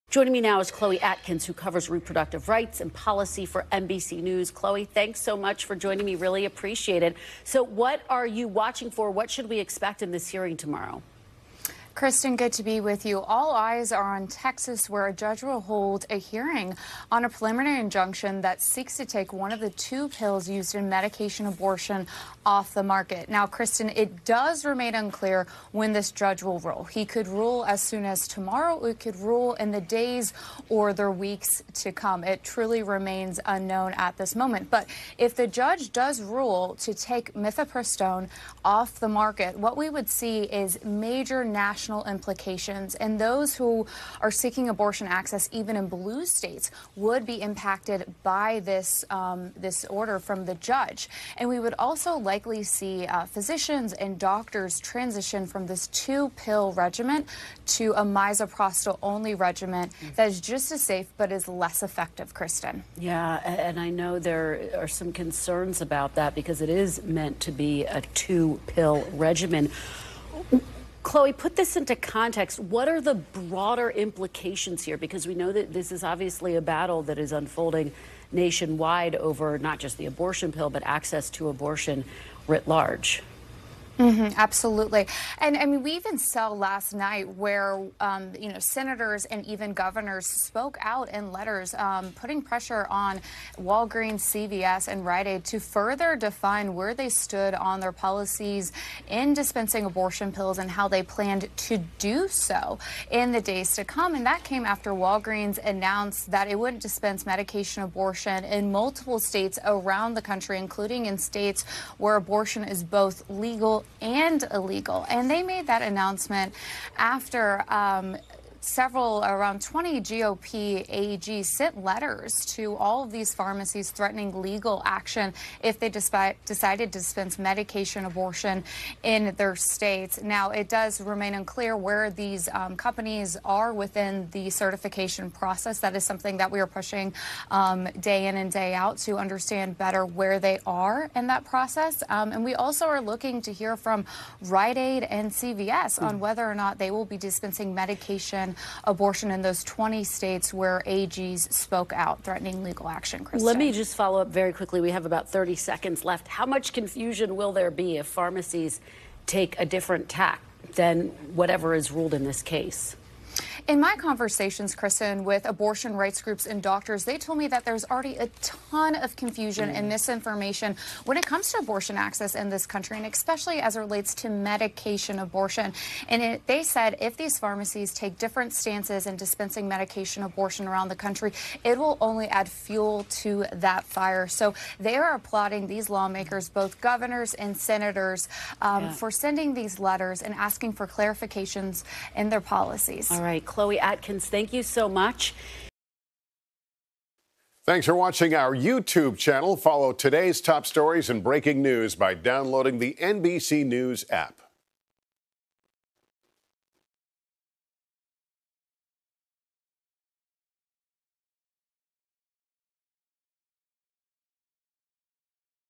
Walgreens, other pharmacies ‘add fuel’ to abortion pill confusion
A Texas judge in a high stakes case over abortion pills has scheduled the first hearing in the case for this week as Democratic lawmakers asked major pharmacies to clarify their plans for dispensing the medication. NBC News Correspondent Chloe Atkins discusses the nationwide implications of the impending decision.